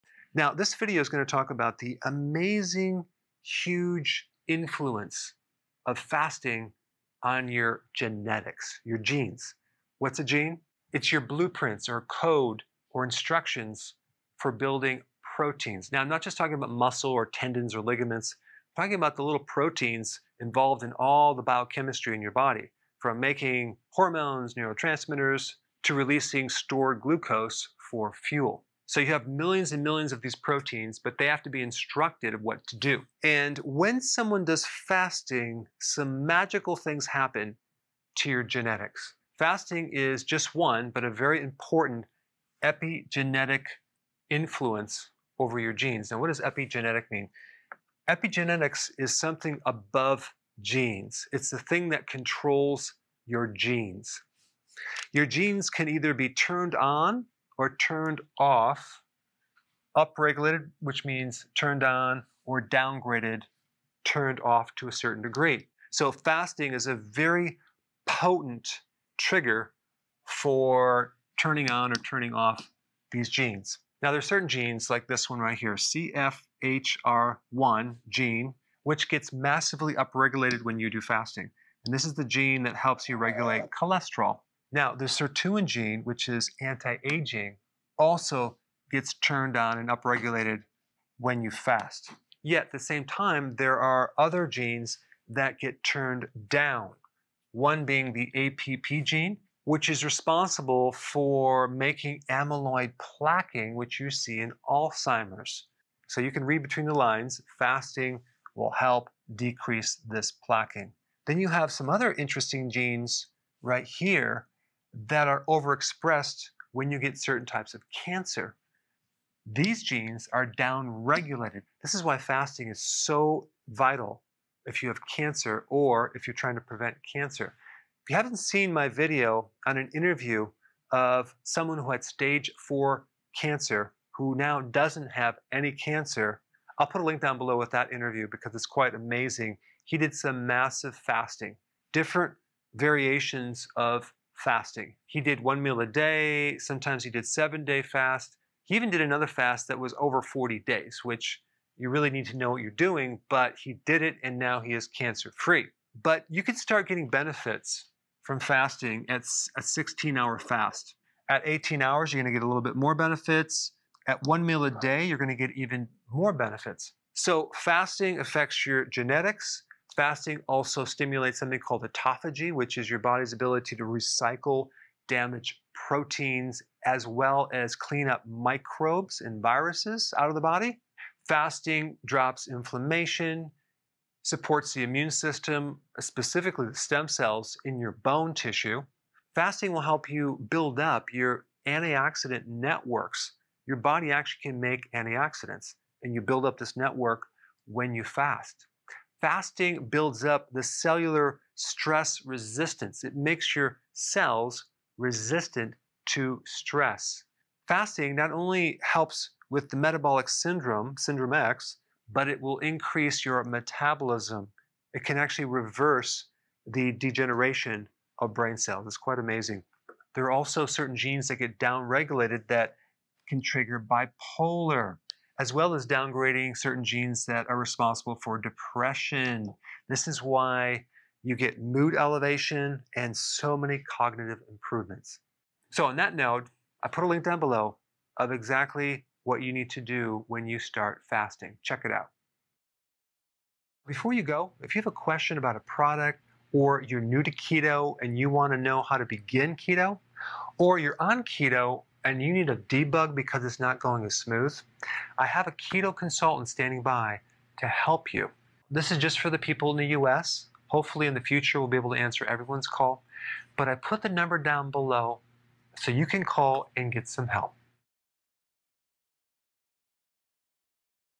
How Fasting Influences Your Genes
Did you know that fasting influences your genes? Here’s how.

Timestamps
0:00 How fasting influences our genes
0:10 What are genetics and epigenetics?
1:05 Fasting and genetic changes
2:36 Fasting benefits
4:03 Need keto consulting?

In this video, we’re going to talk about how fasting influences our genetics.

Your genes are your blueprints or your code for building proteins. These proteins are involved in all of the biochemistry in your body—from making hormones and neurotransmitters to releasing stored glucose for fuel.

You have millions upon millions of these proteins, but they have to be instructed on what to do.

When someone does fasting, many interesting things happen to your genetics. Fasting is one very important part of your epigenetics.

Epigenetics are things that influence your genes. Your genes are like a series of light switches—they can be turned on and turned off. Epigenetics has the ability to turn on and off each light switch.

As an example, fasting positively influences the gene CFHR1, which regulates your cholesterol. It also influences the SIRT-3 gene, which has anti-aging potential.

Fasting also down-regulates genes like the APP gene that helps lower amyloid plaque, which is seen in neurodegenerative diseases like Alzheimer’s.

Fasting may also play a role in other genes that influence cancer risks.

You can begin seeing the benefits of fasting after just a 16-hour fast. One meal a day (OMAD) will give even more benefits.

Fasting can help:
• Lower inflammation
• Support your immune system
• Lower syndrome X
• Support healthy brain cells
• Promote a healthy mood and mind

I hope this helped enlighten you on how fasting influences your genetics.